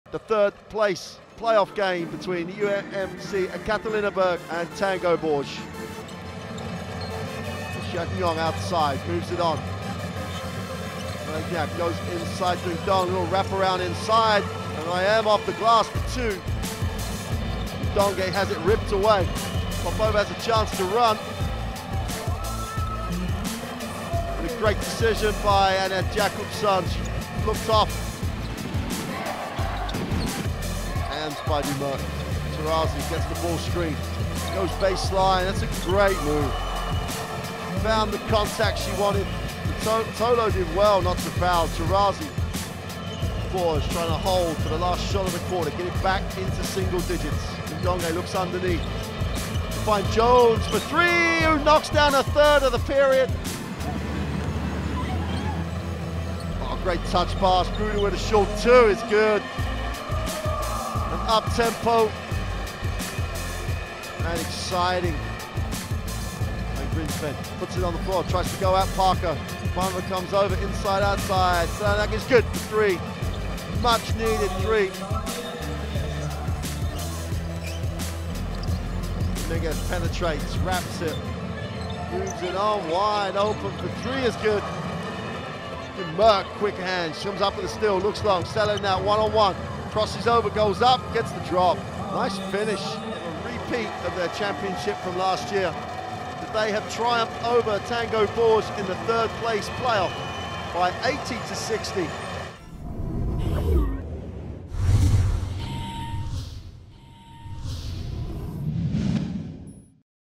F8 Highlights: UMMC Claim Third Spot On Podium
UMMC Ekaterinburg have again defeated Tango Bourges heavily at the EuroLeague Women Final Eight, defeating their French opponents 80-60 to claim third spot on the podium. We've moved! For all new FIBA Europe content please head